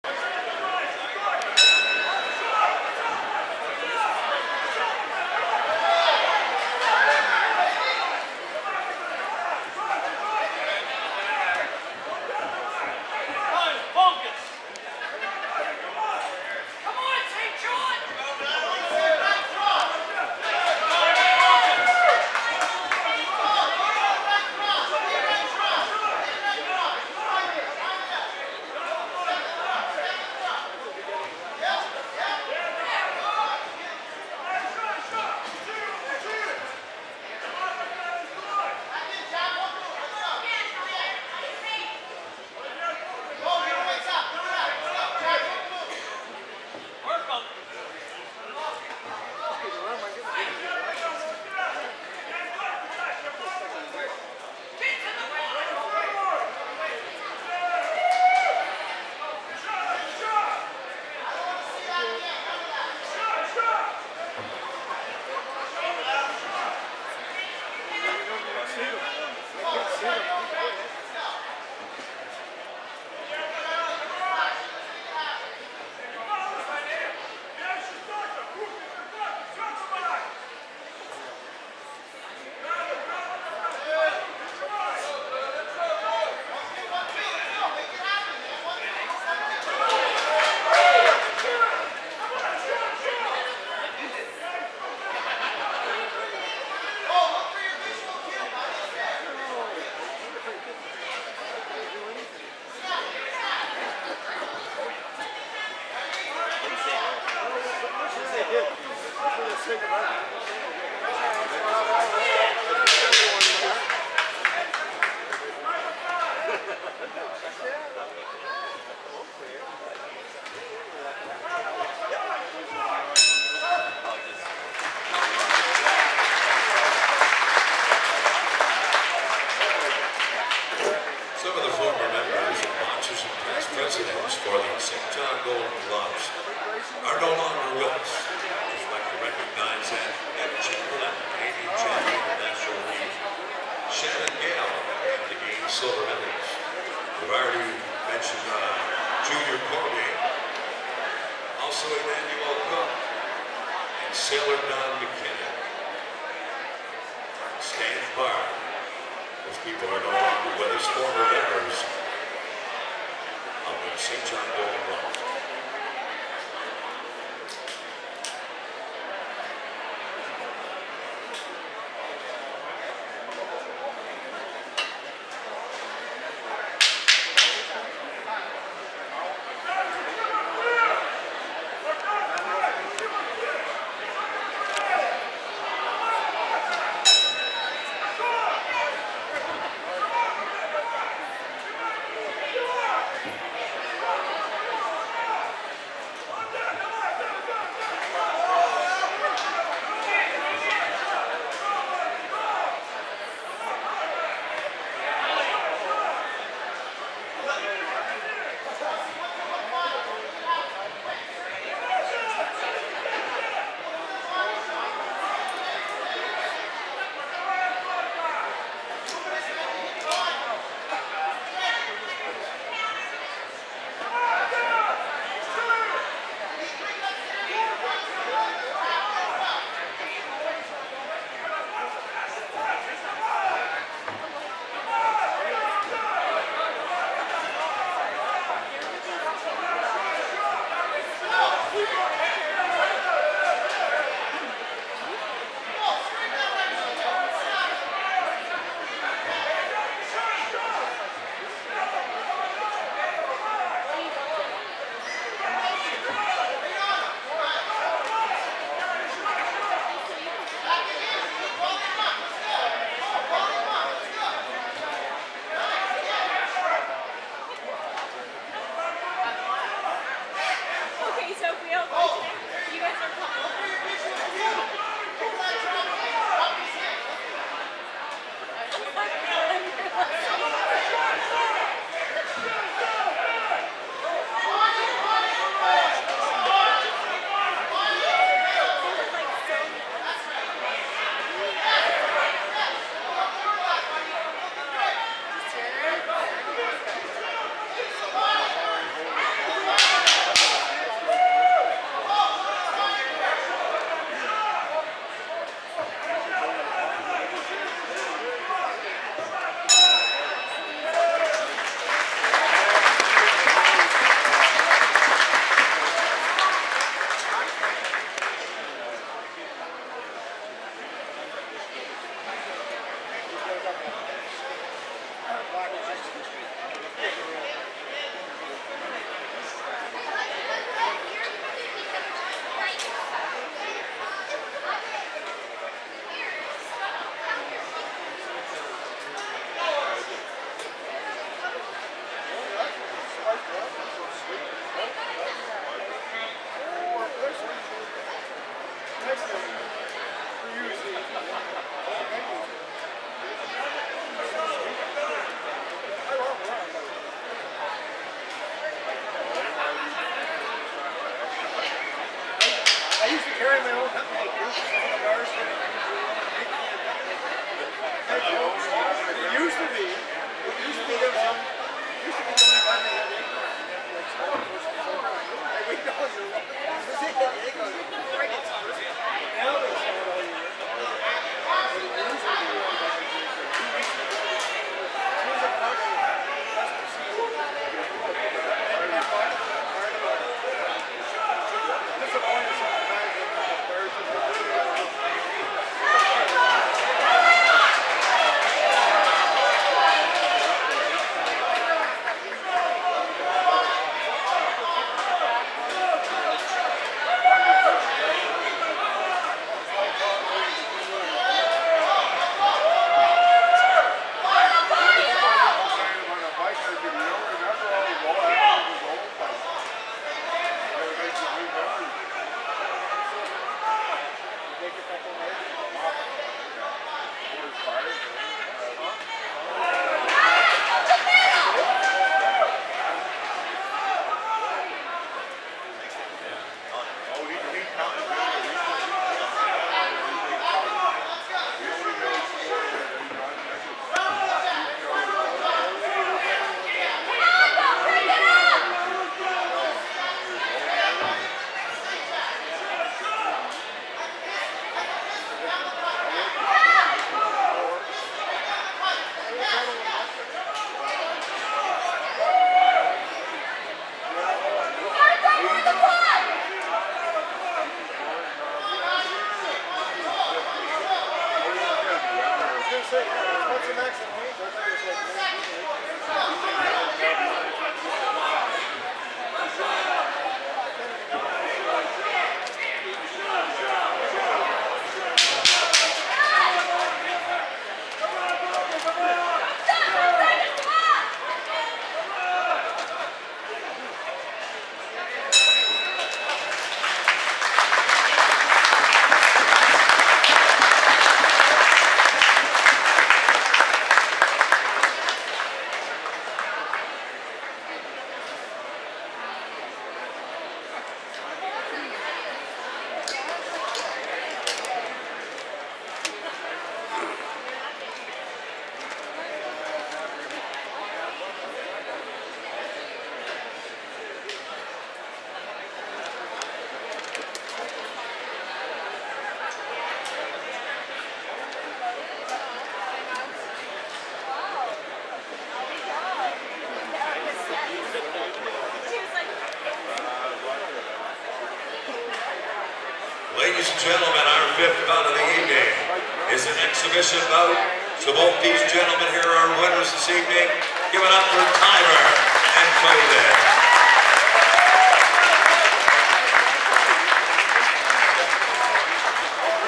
Tivor Stewart(Red) VS Bohdan Kyslenko(Blue)
Tivor Stewart(Red) from the Albion Boxing Club VS Bohdan Kyslenko(Blue) 2018 Canadian Bronze medalist representing Saint John Golden Gloves at "Fights Under The Lights" celebrating Saint John Golden Gloves 50th Anniversary Nov/17/2018